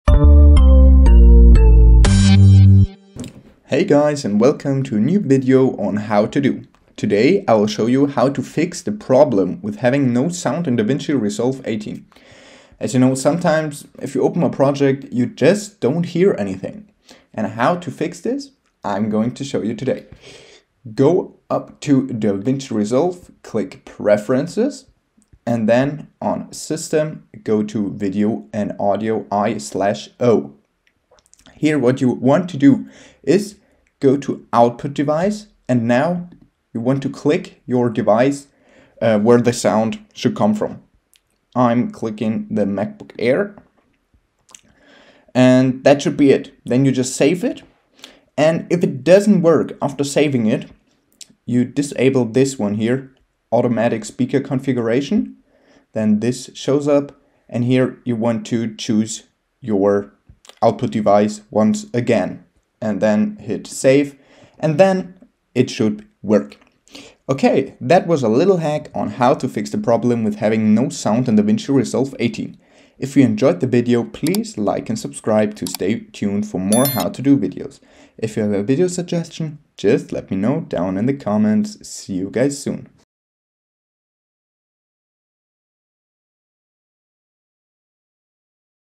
How to Fix the "NO SOUND" Problem in DaVinci Resolve 18
Hey guys,
in this tutorial i will show you how to Fix the "NO SOUND" problem in DaVinci Resolve 18.